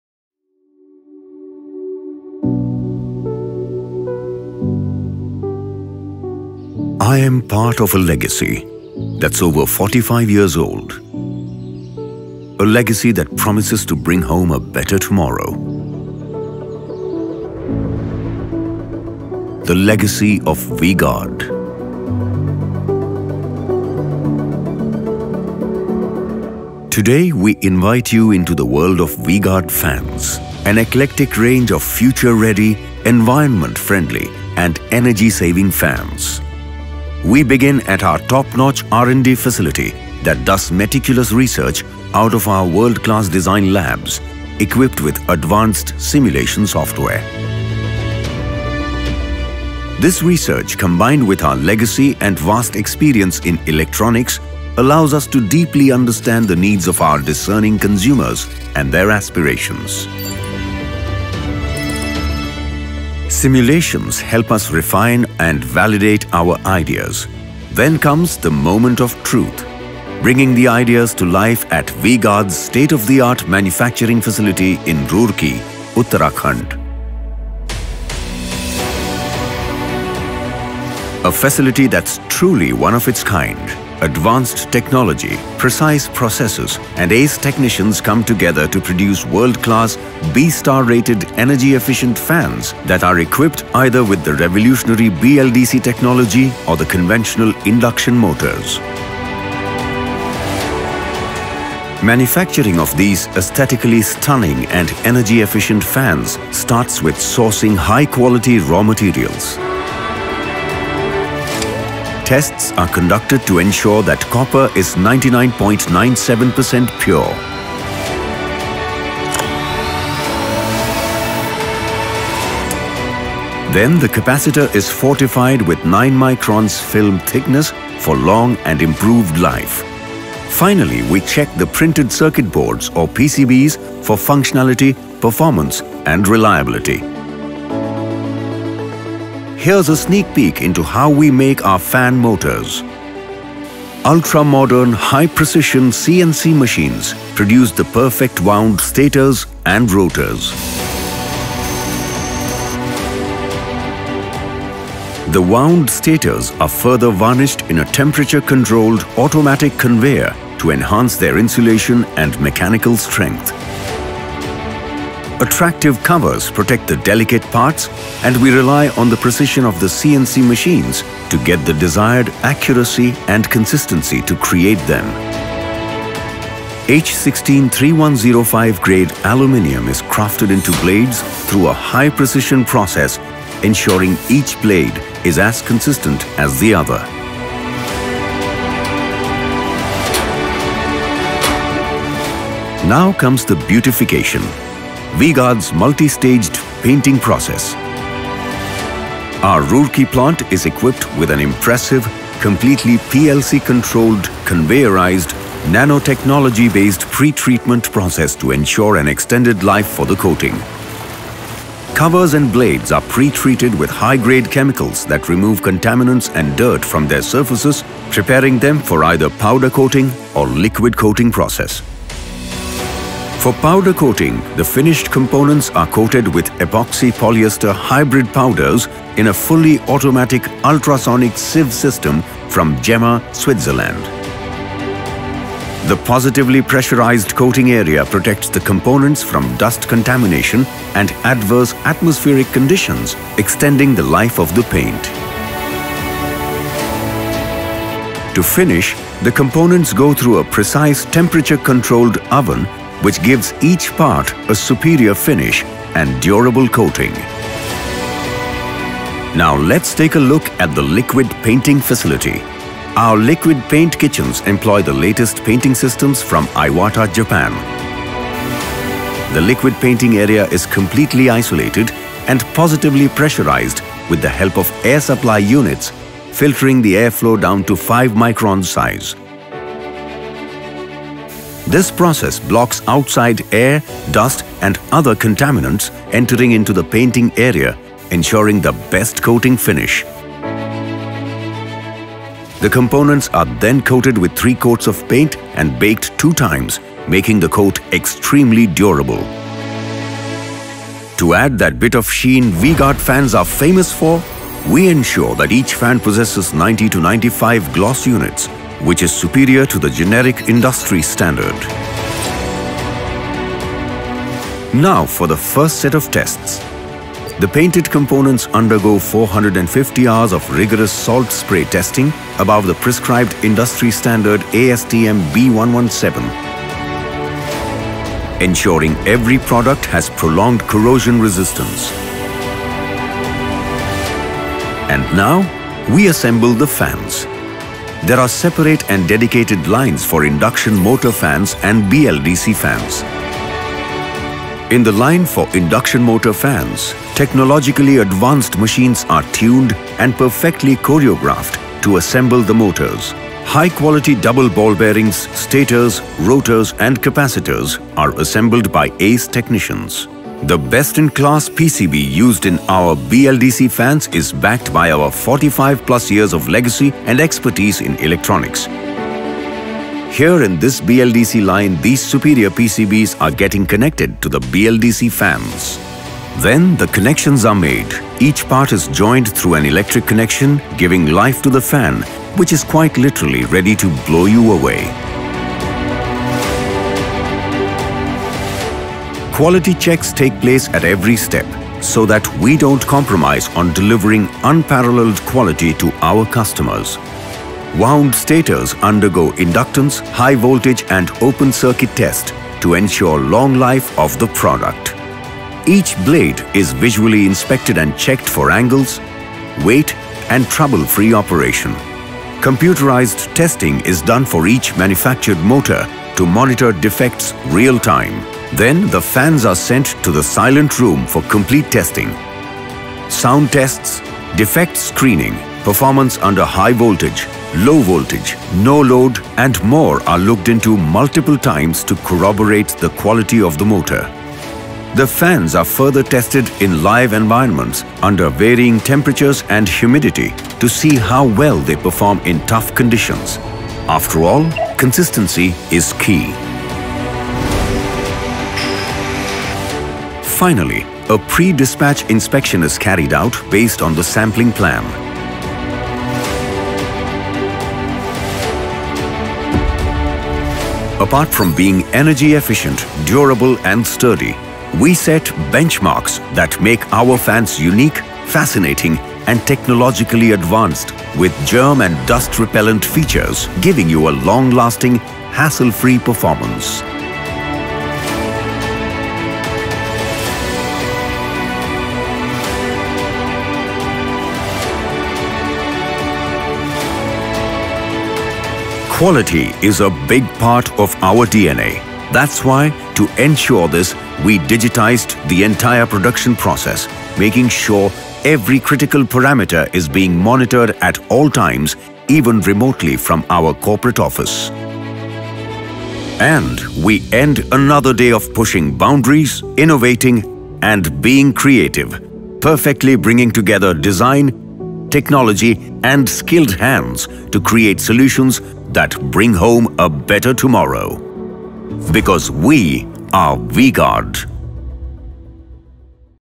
Discover V-Guard's State-of-the-Art Manufacturing Facility for Fans
At V-Guard, every fan is made with meticulous attention to detail, ensuring unparalleled quality and durability. Our state-of-the-art manufacturing facility in Roorkee, Uttrakhand utilizes the latest technology, automation and ace technicians to produce fans that meet the highest standards of performance, looks and energy efficiency.

In this video, you will get an inside look at our manufacturing facility and see firsthand how V-Guard's commitment to excellence sets it apart.